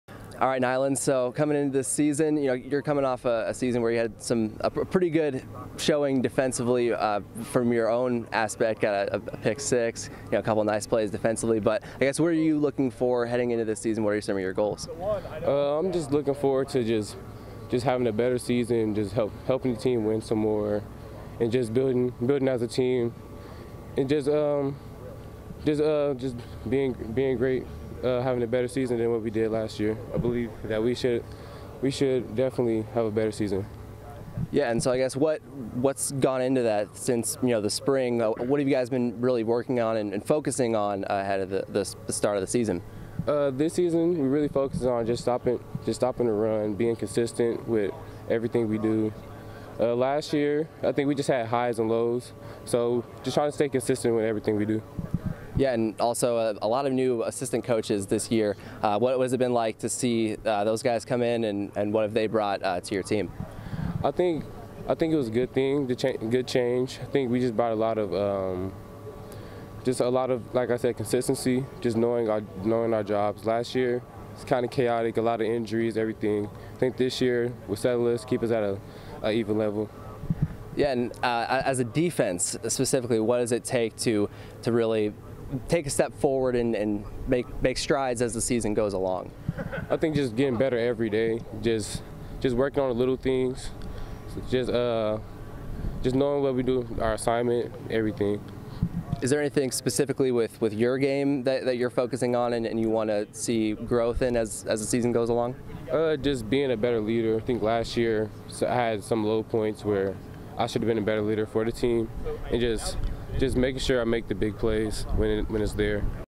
Knylen Miller-Levi aims to lead team improvement for MSU in 2022
The Minot State NSIC Player to Watch wants to step in to a bigger leadership role this season